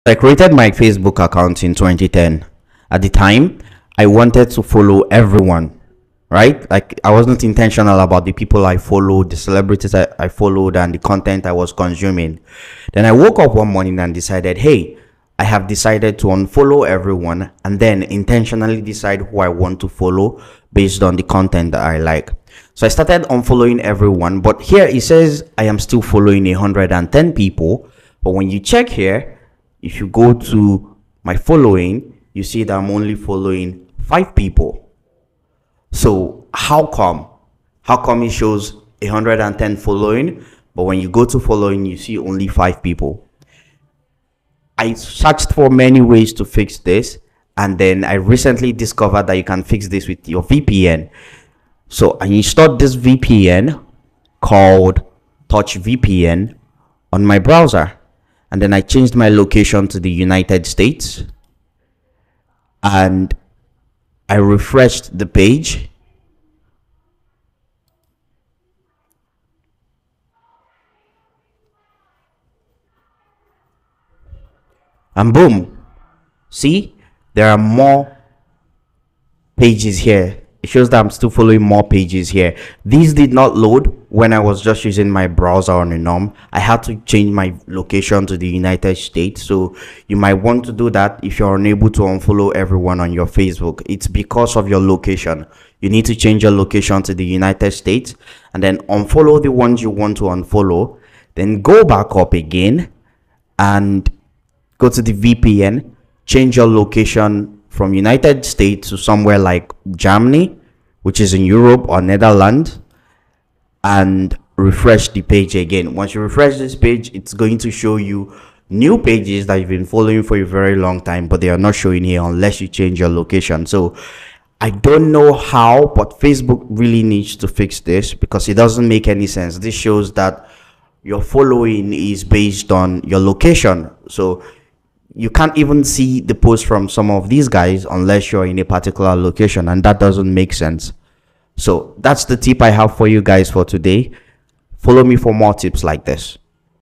Fix Facebook Can't Find Following or Can't Unfollow Everyone | How to Unfollow All
If you are having troubles unfollowing everyone on Facebook then try this